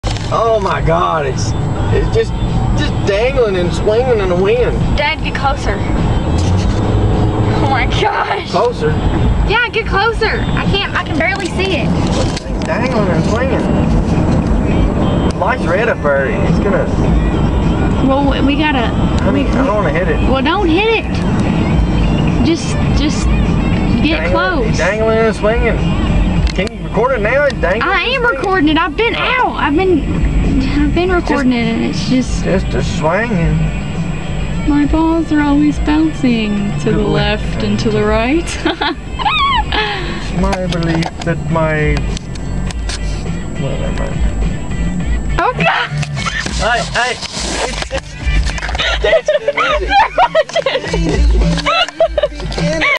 balls!
so there were some balls hanging off that truck. i started laughing at the end because i looked over at the car next to ours and the people on it were looking at me funny and it was great.